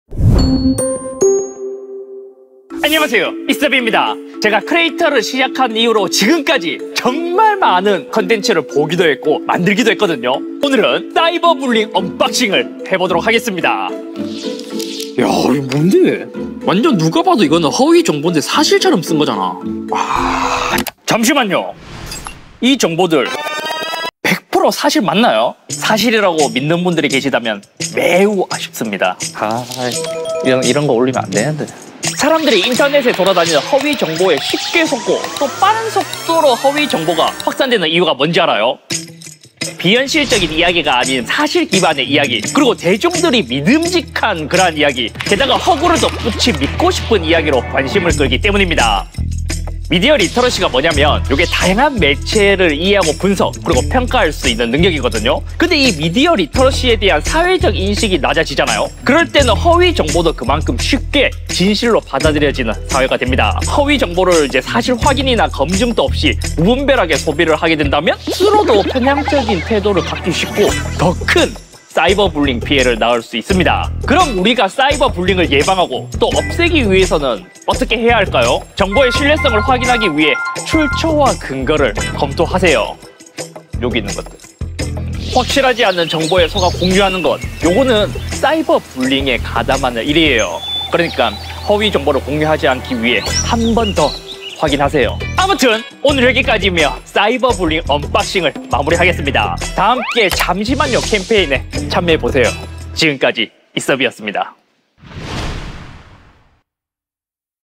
[잠시만요] 사실 확인되지 않은 정보 공유는 사이버불링일 수 있어요
건강한 인터넷을 만들기 위해, 지금 잠시만요 캠페인에 참여하세요 👀

⏸️ '잠시만요 캠페인'은 사이버불링(Cyberbullying, 인터넷 상에서 다른 사람을 괴롭히는 행위)에 초점을 맞춰, 사이버불링 예방과 확산을 방지할 수 있는 방법을 공유하는 YouTube 캠페인입니다.

💪 모든 콘텐츠는 캠페인의 파트너이자 국내 인터넷 이용자들의 디지털 활용 능력과 소양 향상에 앞장서고 있는 사단법인 디지털리터러시교육협회와 협력하에 기획 및 제작되었습니다.